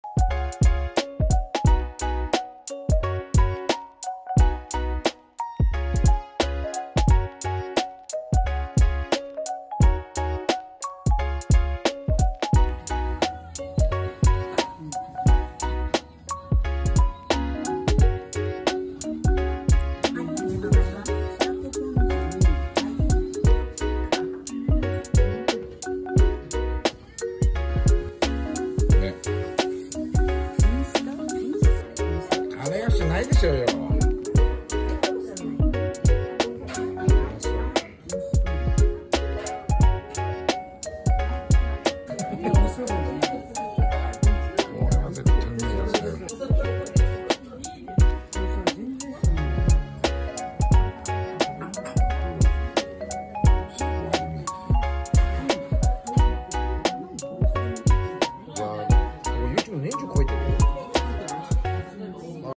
【北千住】ビストロ寛‼️ショート風に何度でも…(o^^o)だって美味しいんだもん何度だって行きますよ〜
本日お邪魔は
前にも紹介しています
住宅街の隠れた名店
ビストロ寛さん‼️
駅から少し歩きますが
何を食べても美味しく
それまでの労力を
全て忘れさせてくれるほど
何度だって紹介しますよ〜笑
絶対満足感が味わえると思います
是非ともお試しあれ〜(o^^o)

和食ビストロ寛
東京都足立区千住寿町13-11 1Ｆ

達人にご意見ご依頼の方はこちら
お問合せお待ちしております

 @sibainu-haku と
 @akanecollage も
北千住の達人を応援しています‼️